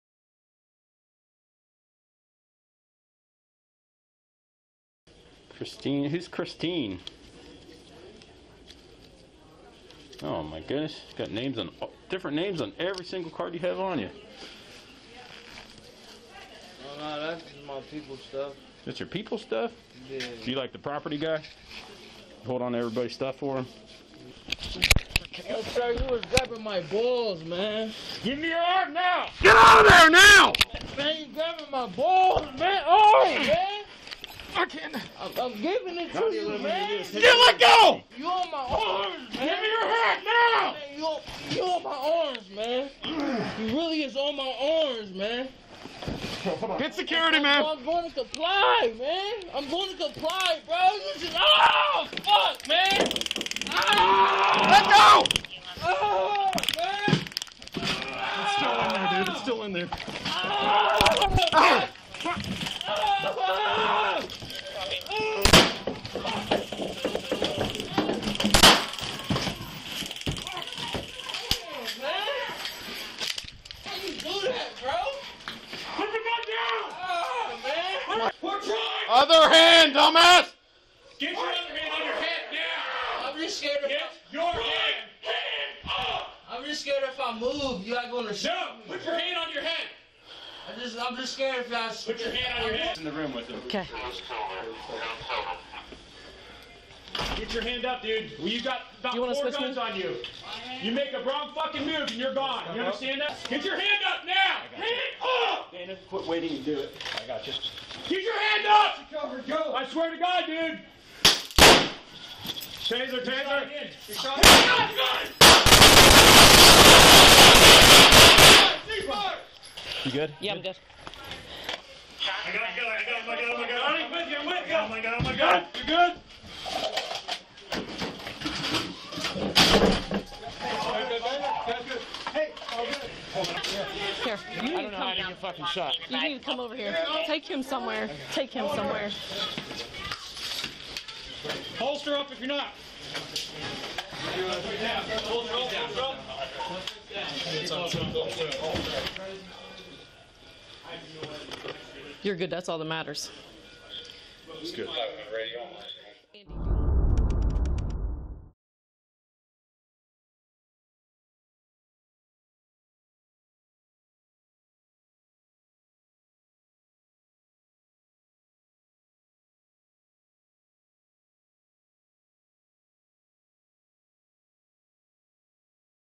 Una cámara graba la muerte de otro joven afroamericano tiroteado por varios policías en un hospital
Otro joven afroamericano ha muerto por los disparos de la policía en Estados Unidos. Esta vez ha ocurrido en un hospital de Columbus, Ohio. Las imágenes pueden herir su sensibilidad.